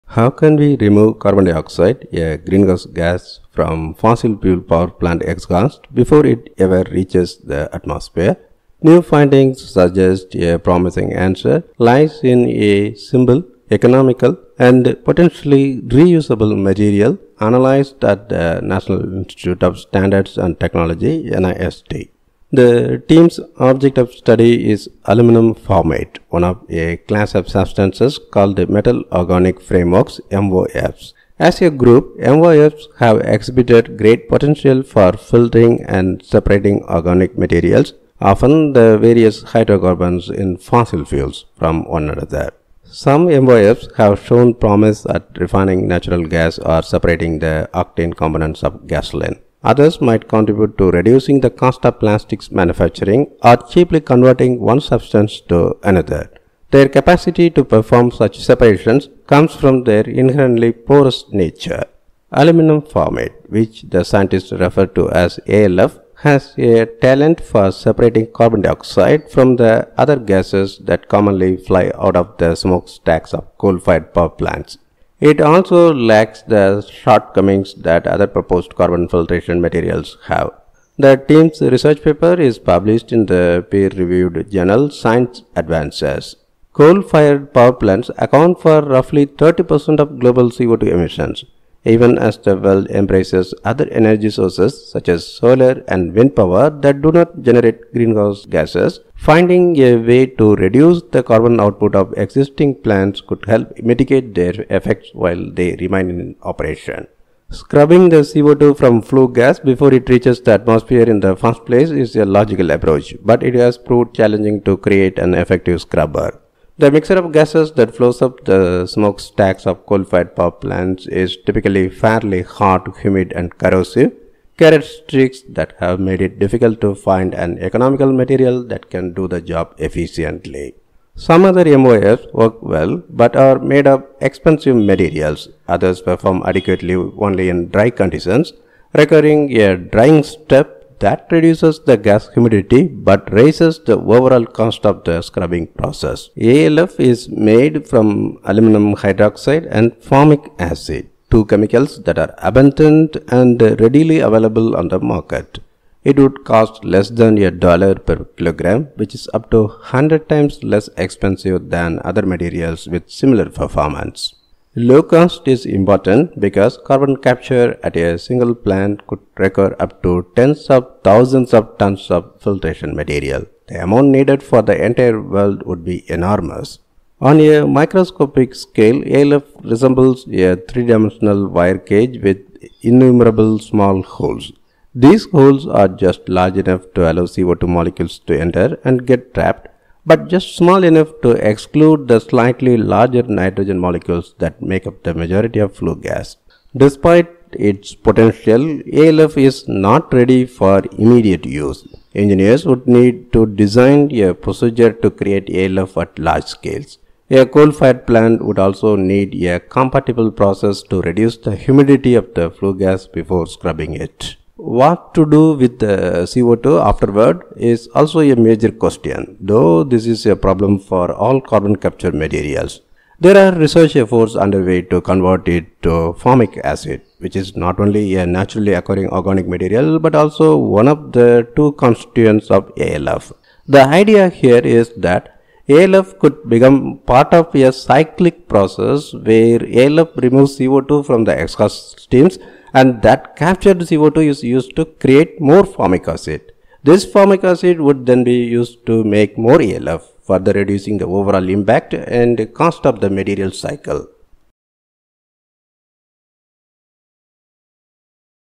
This simple material could scrub carbon dioxide from power plant smokestacks | MOF Aluminum Formate
An easily synthesized chemical filter could stop the greenhouse gas from reaching the atmosphere.

Credit: B. Hayes / NIST

Research Paper: Aluminum formate, Al(HCOO)3: An earth-abundant, scalable, and highly selective material for CO2 capture